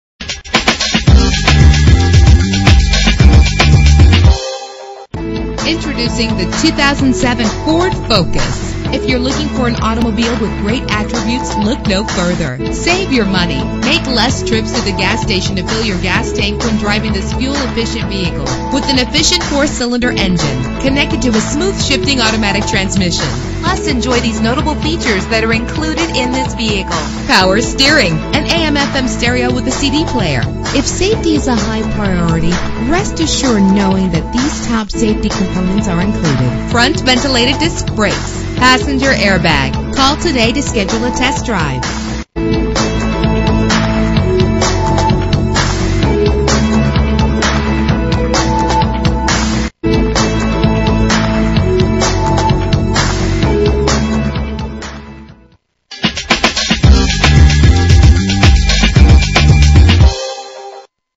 2007 Ford Focus - Scarsdale NY
Year: 2007
Make: Ford
Model: Focus
Trim: SE
Engine: 2.0 liter inline 4 cylinder 16 valve
Transmission: 4-Speed Automatic
Color: White
Mileage: 29170
Address:
887 Central Park Avenue